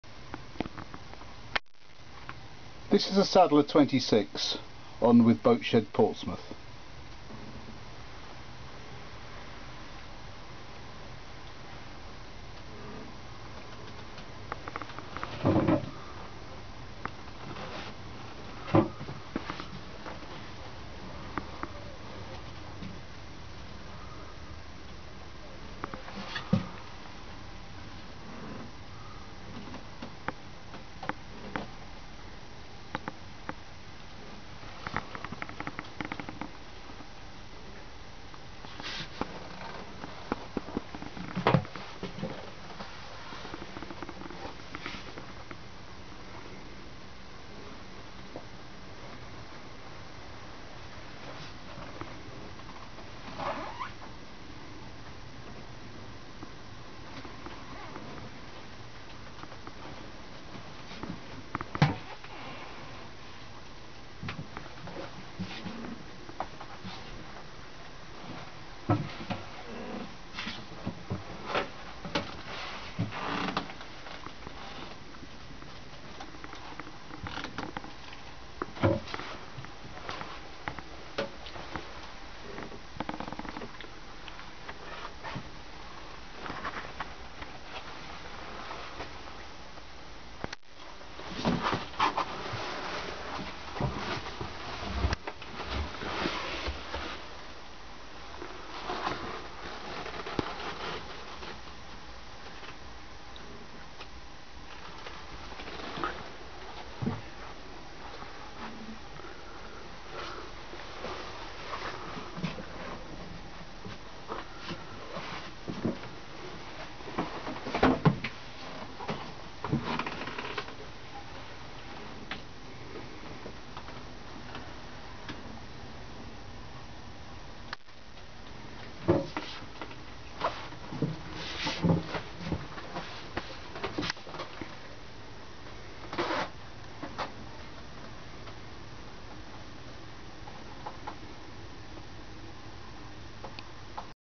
Sadler 26 - Boatshed.com - Boat Ref#172122
Sadler 26 for sale with Boatshed Portsmouth Photos and video taken by Boatshed Portsmouth